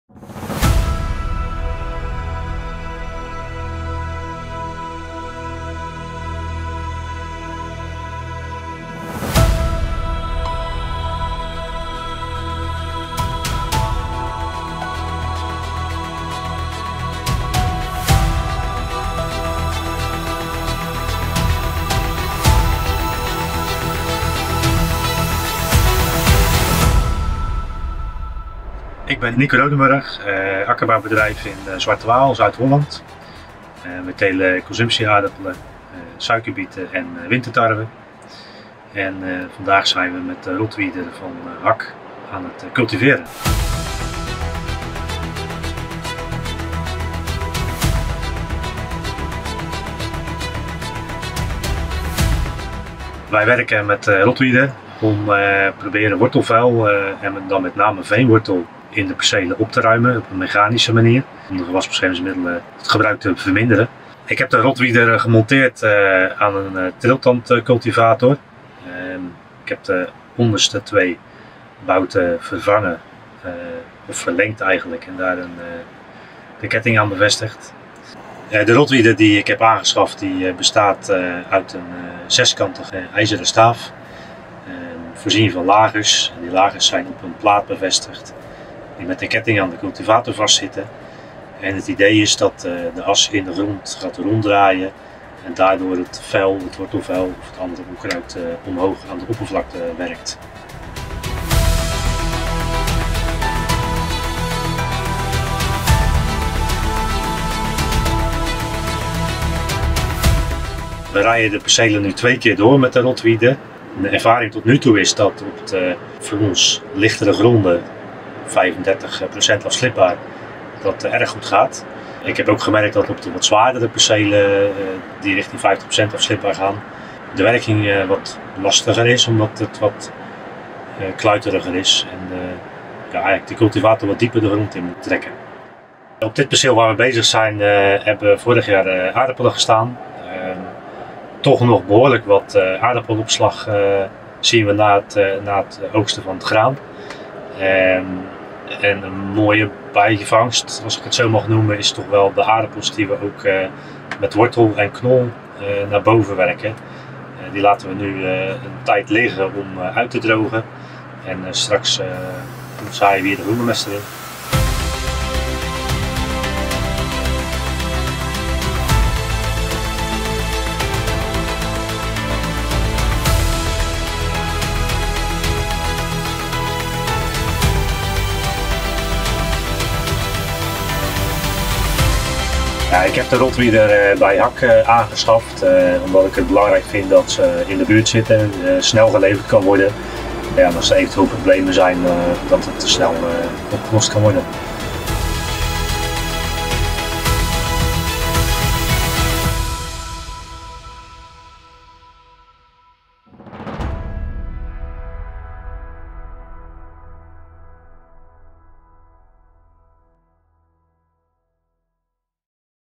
Nico Rodenburg legt uit hoe de HAK RodWeeder werkt
Nico Rodenburg van akkerbouwbedrijf Brielse Boer in Zwartewaal bindt de strijd aan met diep wortelend onkruid als veenwortel. Hij gebruikt daarvoor de HAK RodWeeder, die hij aan zijn triltandcultivator heeft bevestigd. Hoe het werkt legt hij uit. (met Nederlandse of Engelse ondertiteling naar keuze)

Nico Rodenburg of the Brielse Boer arable farm in Zwartewaal is tackling deep-rooted weeds like peat root. He uses the HAK Rod Weeder, which he has attached to his vibrating tine cultivator. He explains how it works. (with Dutch or English subtitles at your choice).